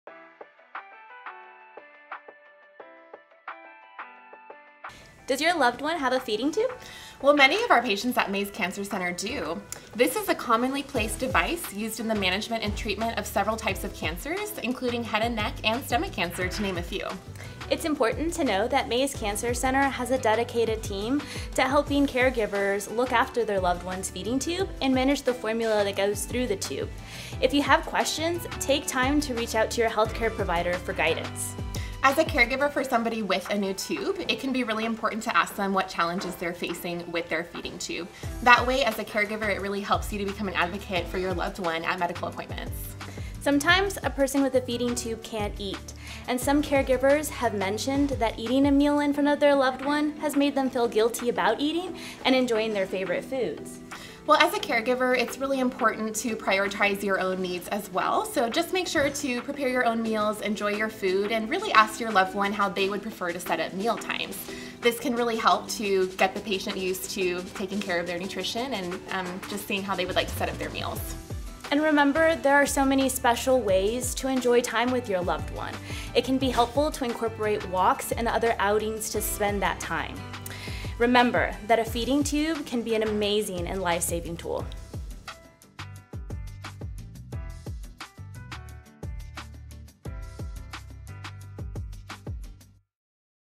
Feeding tube tips
Some caregivers might feel guilty about eating in front of their loved ones who use feeding tubes. During WorldCancerSupportMonth, our clinical dietitians at UT Health San Antonio MD Anderson Cancer Center want to share tips with those providing nutritional care to their family or friends who have a cancer diagnosis.

It's important to remember to take care of yourself, too. Have open conversations with your loved one about how they are most comfortable with mealtime.

Thank you to Tracy and Taryn for these tips!